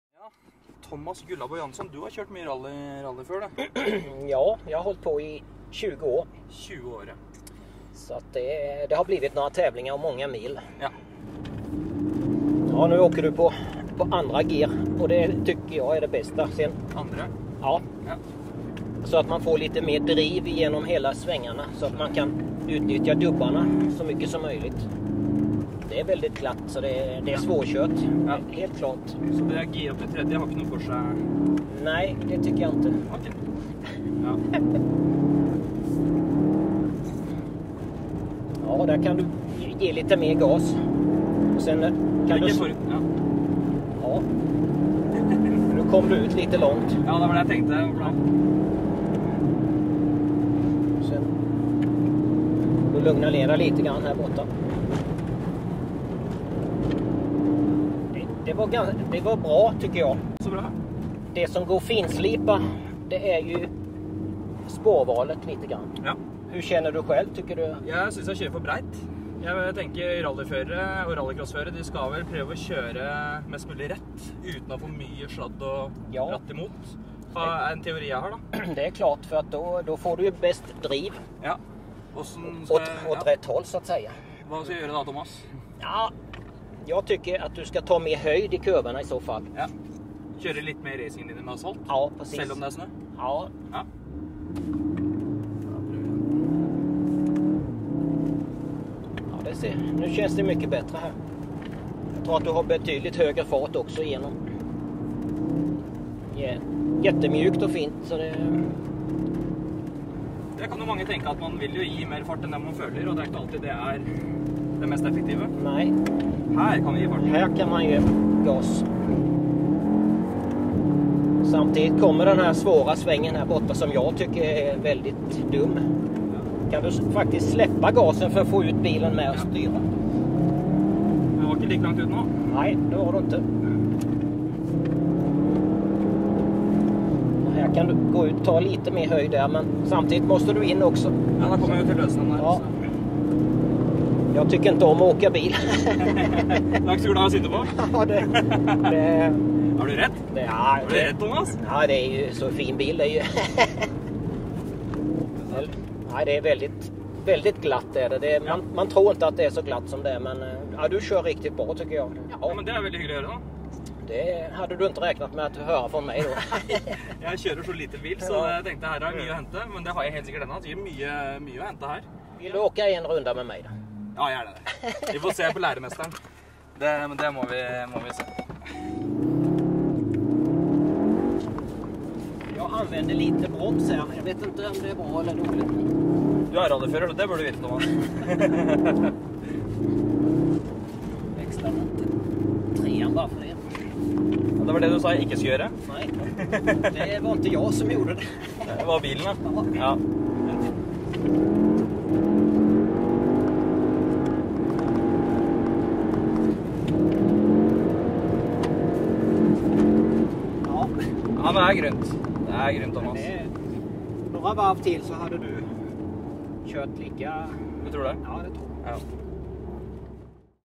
VW Golf R på Golsfjellet med Thomas «Gullabo» Jansson
– Jag tycker inte om åka bil, sier den svenske rallylegenden Tomas «Gullabo» Jansson når han skal forsøke å lære Parc Fermé den optimale kjøreteknikken på løst underlag.